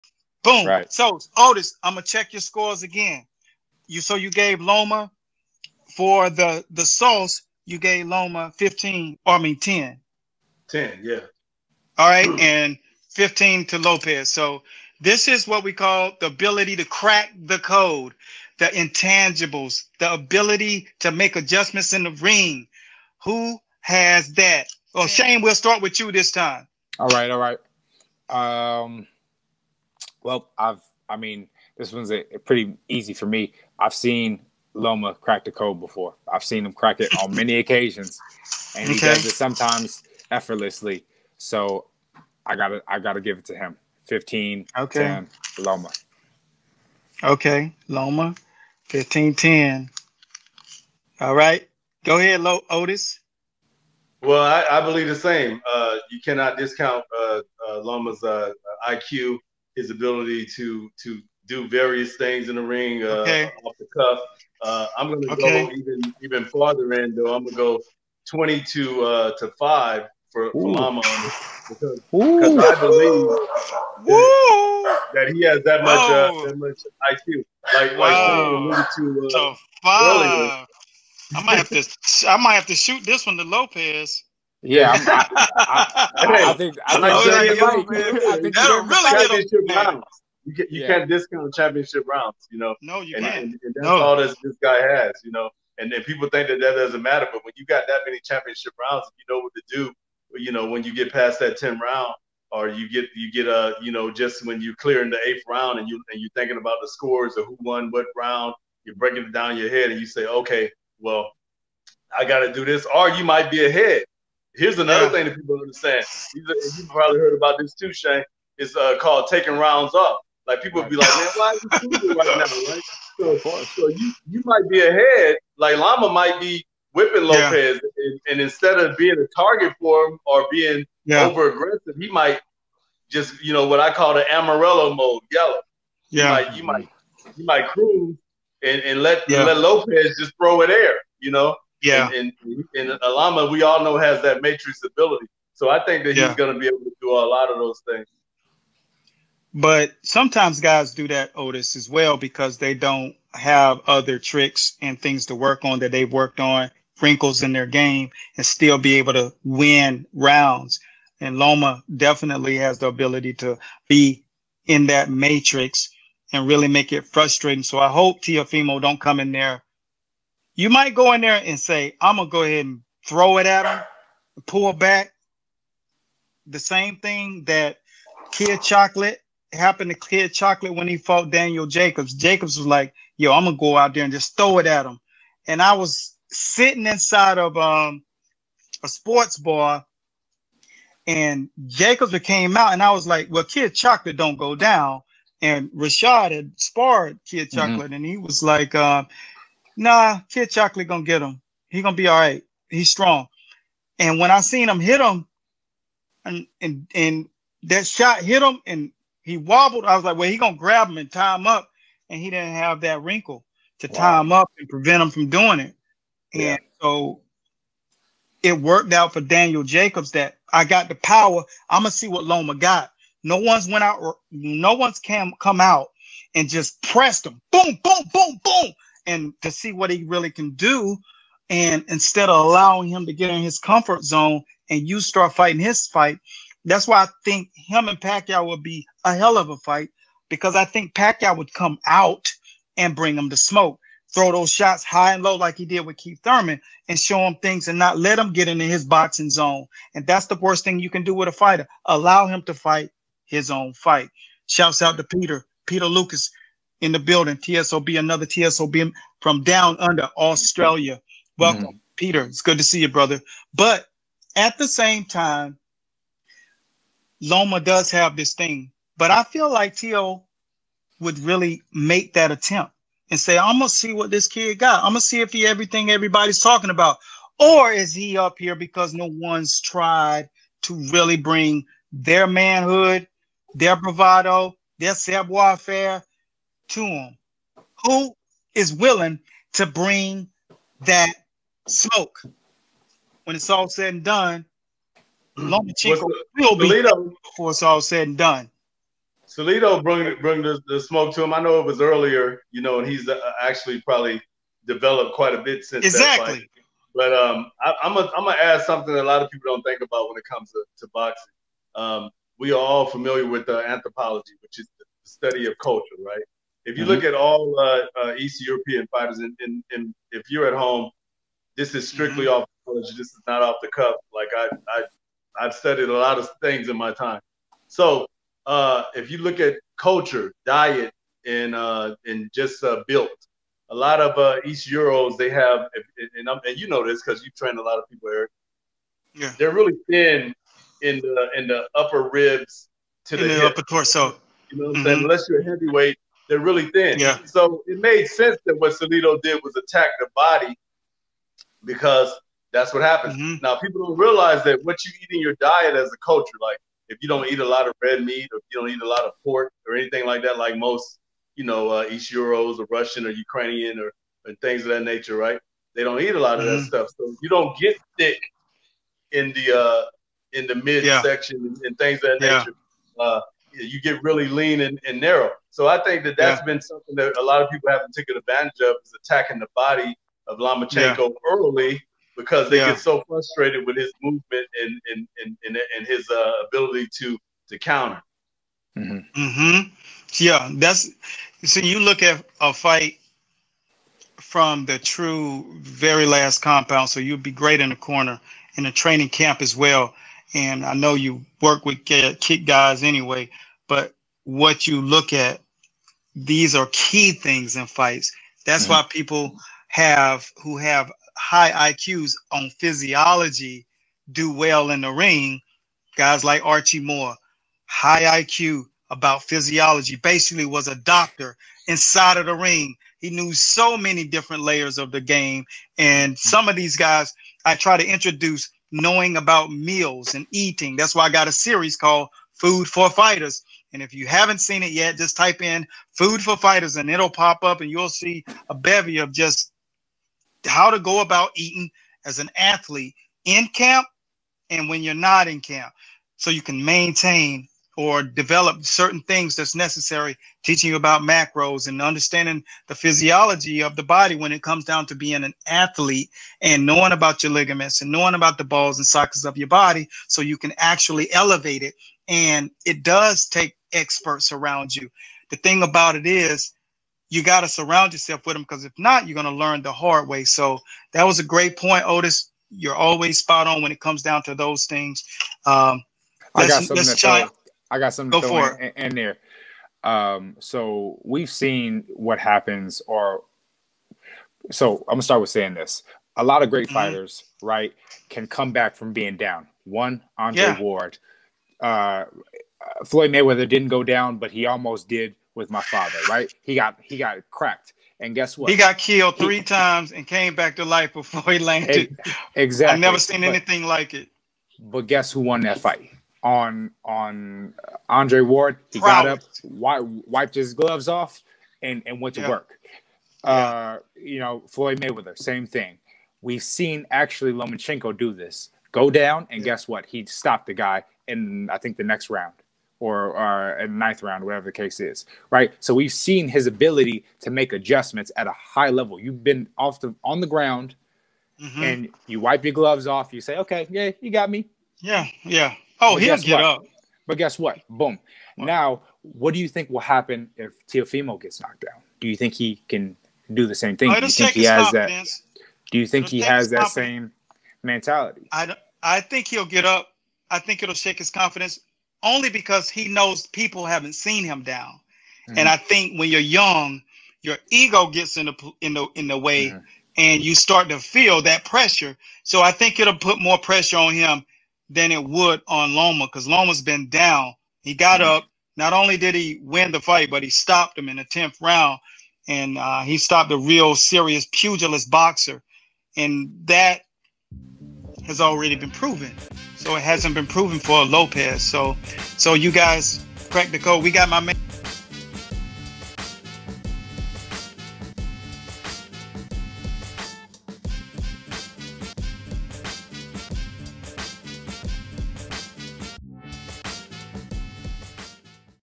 LOMA VS LOPEZ ESPN TOP RANK BOXING | Predictions | Who can crack the code | Weighin
Lomachenko vs Lopez has many components. Here we do a breakdown over several episodes to give you as a boxing fan clarity o how to choose a winner. Lomachenko vs Lopez will be a test of skills and will. See here Ep. 4 where we breakdown who actually make adjustments during the fight.. We title this episode Cracking THE CODE!   Lomachenko vs Lopez.  We're not just talking, we have a world class Boxing Podium and our channel teaches Boxing skills on many levels. Check below for more details.